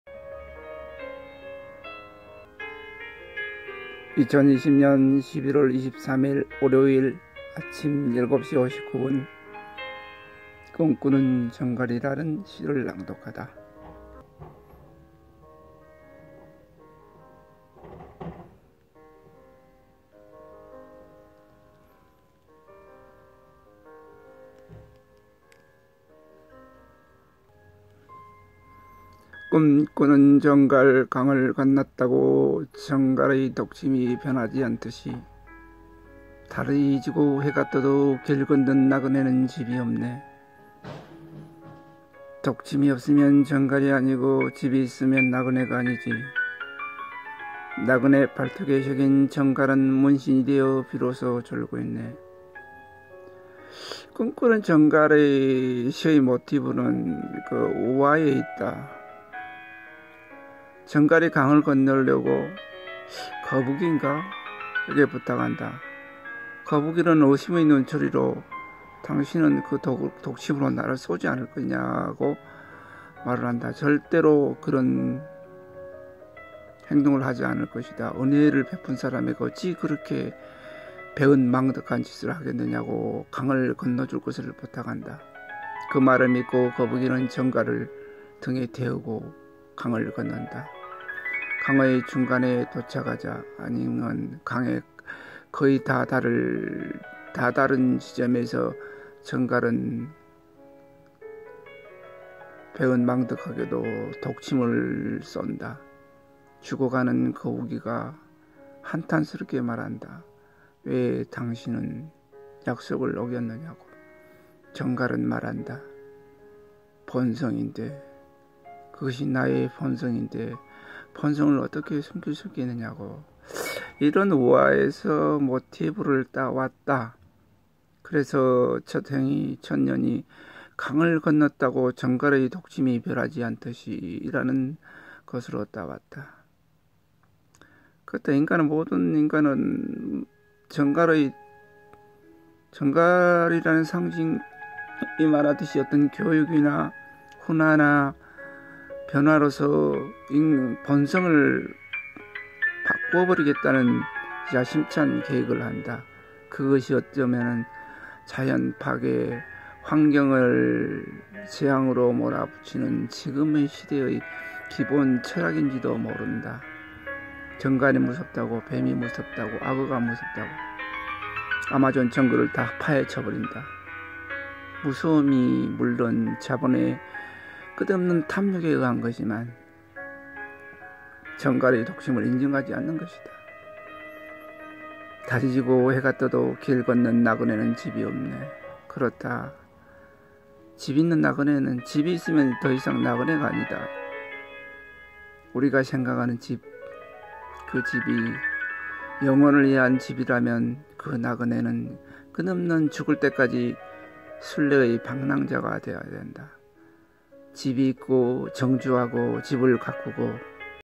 꿈꾸는 전갈 -2
꿈꾸는 전갈
강을   건넜다고
전갈의  독침이  변하지 않듯이
달이 지고 해가 떠도
길 걷는  나그네는
집이  없네
독침이 없으면  전갈이 아니고
집이  있으면   나그네가 아니지
나그네  팔뚝에 새긴
전갈은  문신이 되어
비로소
졸고 있네